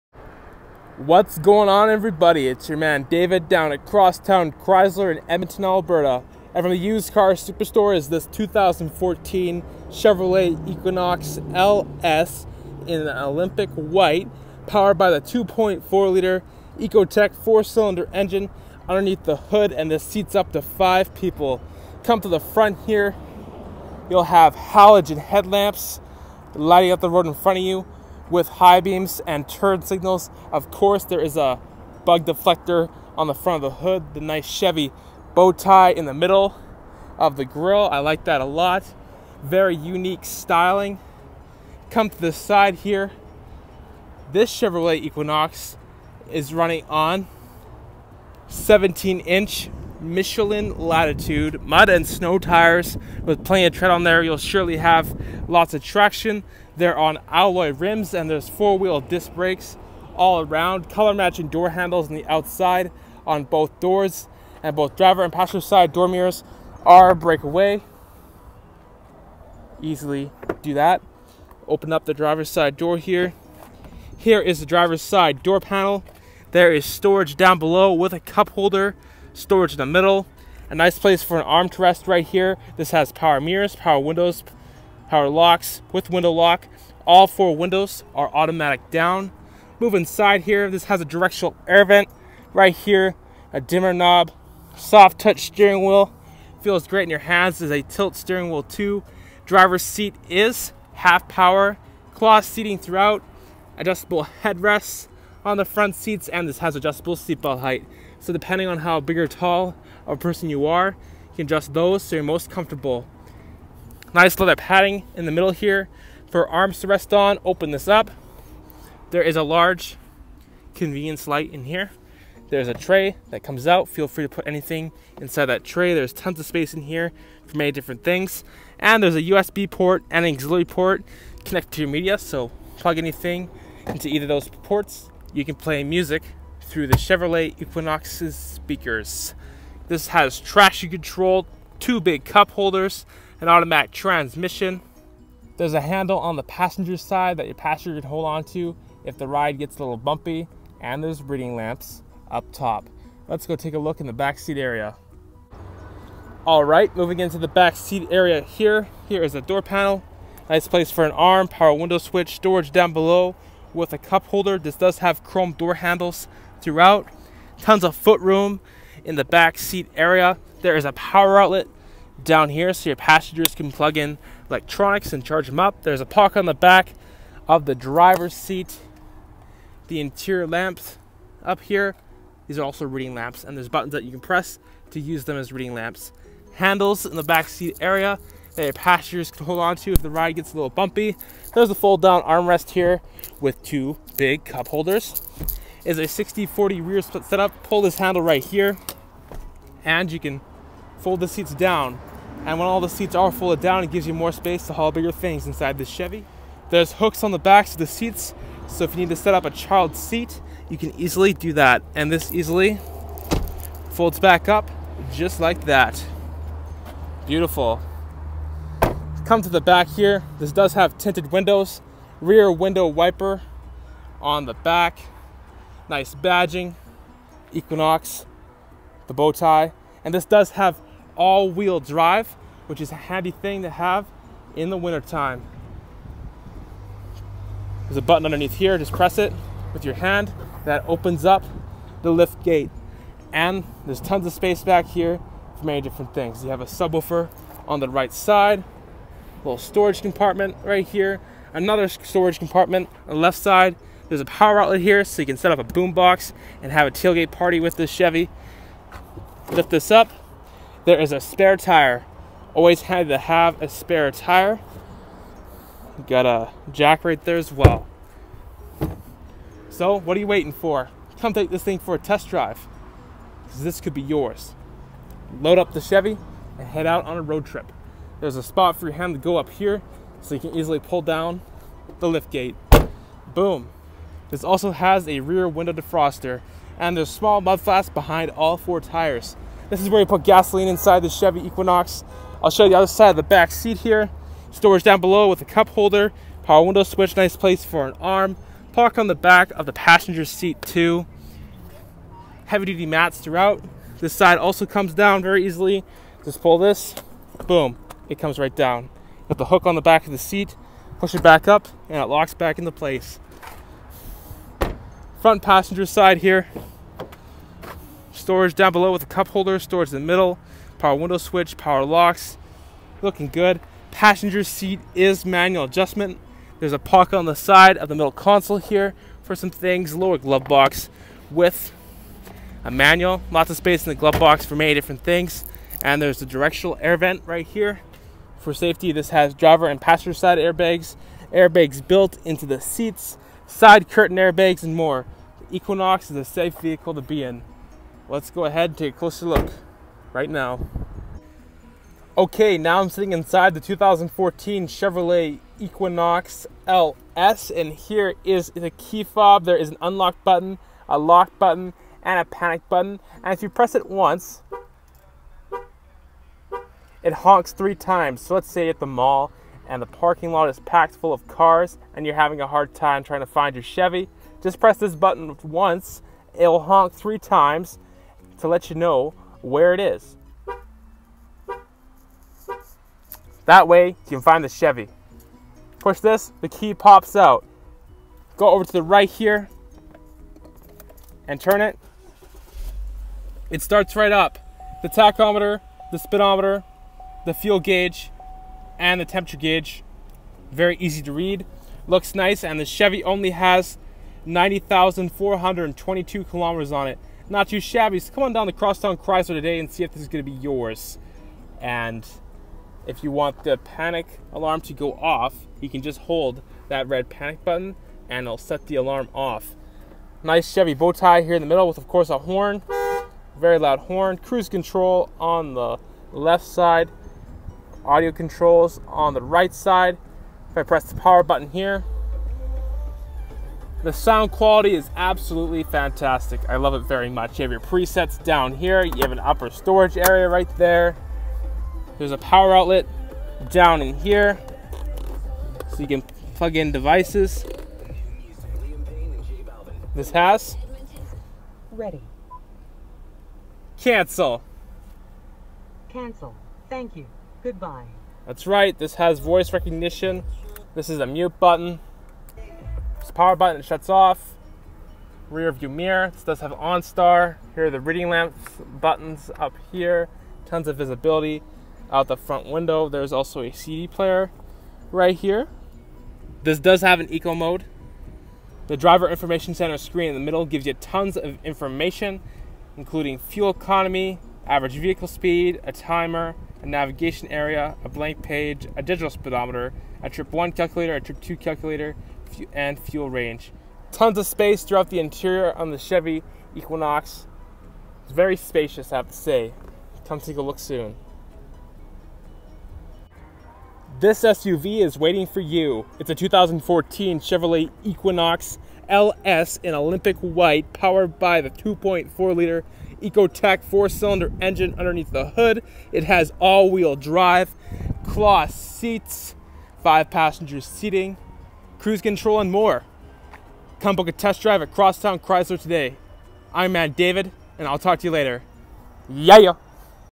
2014 Chevrolet Equinox LS AWD | 2.4L EcoTec Engine | Edmonton AB | 17JR5186A | Crosstown Chrysler
Olympic White, All Wheel Drive, Automatic Headlamps, Eco Mode, Traction Control, Reading Lamps, Power Windows + Locks + Mirrors, Keyless Entry, Tilt Steering Wheel, Cruise Control, Driver Information Centre, OnStar Communication, AM/FM/CD Player/Voice Recognition, Air Conditioning, Child Security Lock and more. Come on down to Crosstown Chrysler in Edmonton AB and check out this 2014 Chevrolet Equinox LS! Underneath the hood is the 2.4L EcoTec 4 Cylinder Engine putting out 182 horsepower, it's a fuel efficient motor that gives out excellent fuel economy. The driver's seat is half power and half manual adjustment, it's very easy to adjust and can be set in many different ways for the best driving position. The Equinox is a solid SUV that is spacious and suitable for families. When driving this Chevy, you'll fall in love with it's quiet and refined ride! Don't wait for too long, this 2014 Chevrolet Equinox LS AWD is looking for a new home, come and take a closer look soon.

Video Voiced & Recorded By, David McIntyre.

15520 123 Ave NW, Edmonton, AB T5V 1K8